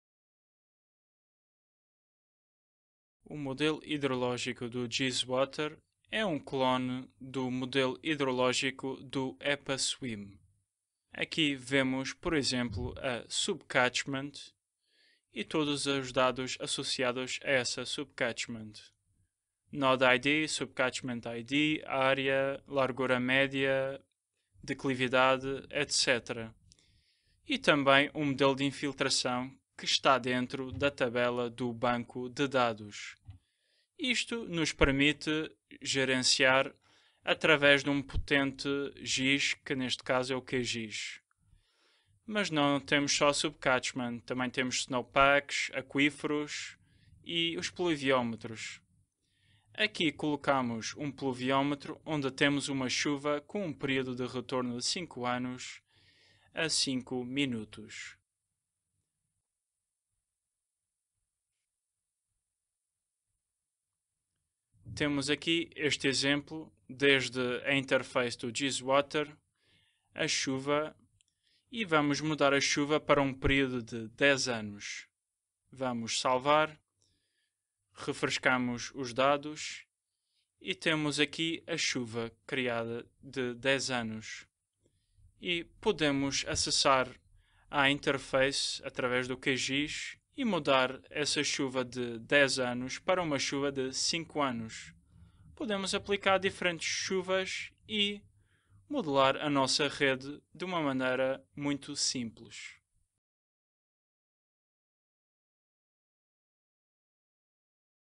Giswater: hidrologia rede de esgotos e drenagem urbana
Com o Giswater, podemos modelar a hidrologia de uma rede de esgotos e drenagem urbana através de um SIG